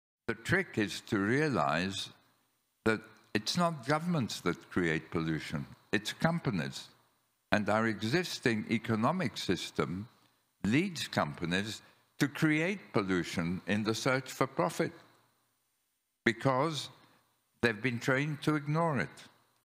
"It's not governments that create pollution - it's companies!" - Sir Ronald Cohen
In conversation with Maurizio Molinari of la Repubblica, Sir Ronald Cohen explains why our current system of capitalism encourages companies to ignore 'externalities' such as their impacts upon the environment. Hence, we need to reshape capitalism to place impact and risk alongside return.
Sir Ronald Cohen is a preeminent international philanthropist, venture capitalist, private equity investor, and social innovator, who is driving forward the global impact revolution.